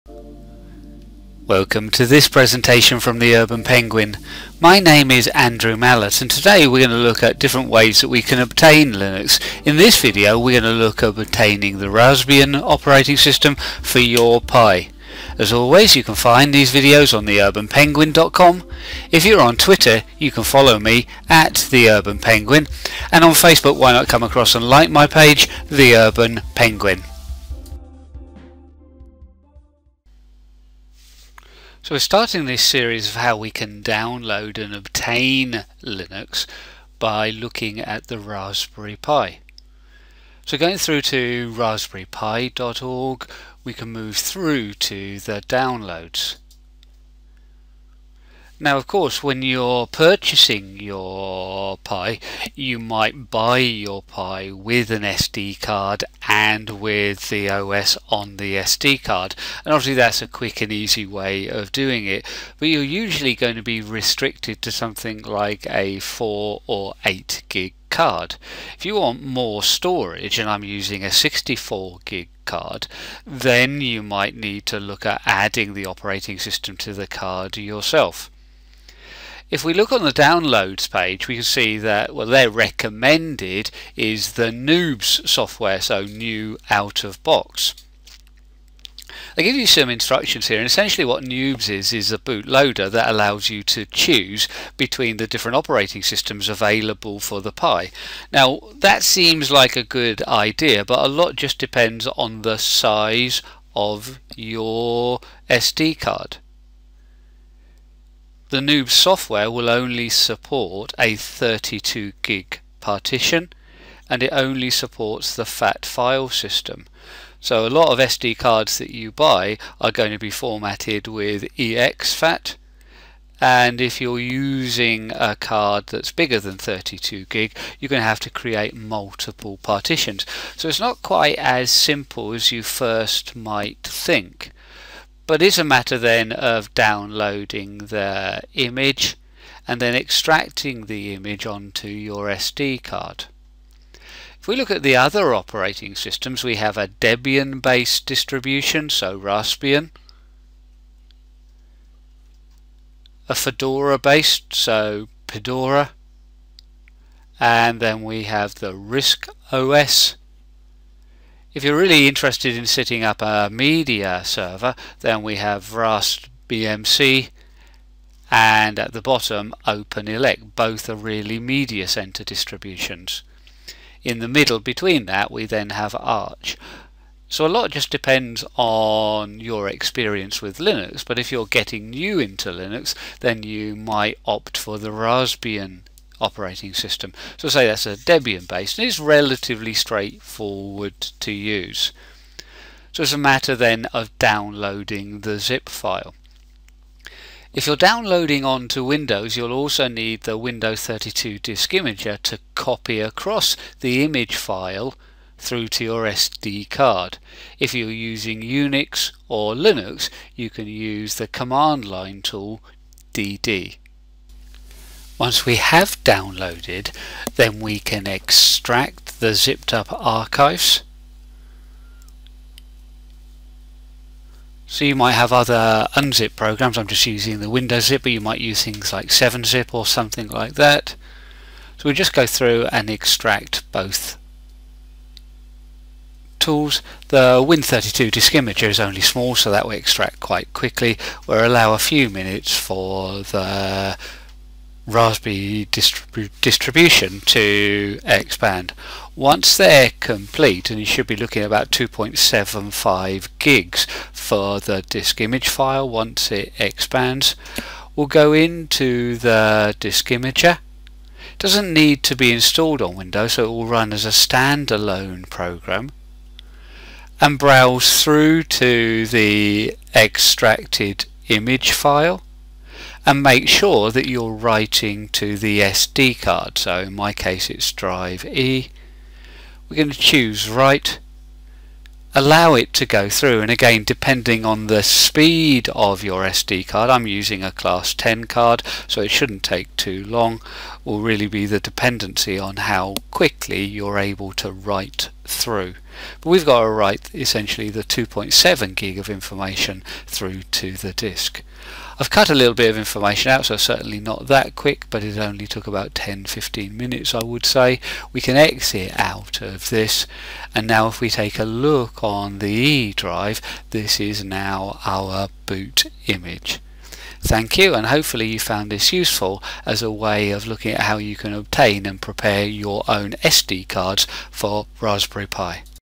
Obtaining Linux : For the Raspberry Pi
If you but the Pi without an SD card you leave yourself the choice of the speed and size of card that you can use. I have a class 10, so is faster and 64 GB SD card to use. NOOBS may seem like a good choice as it is recommended on the download site but only supports 32GB cards and not FATex. You could create two new partitions on the card and look at ways of formating the card fro FAT32 but NOOBS is just a way of proving the choice of OS at boot. If you know the ditro that you want and for starters most people would recommend Raspbian then just down load the OS that you want. You will need to use the Win32 Disk Imager linked on the download page if you use Windows or image the file with dd in Linus to the SD card.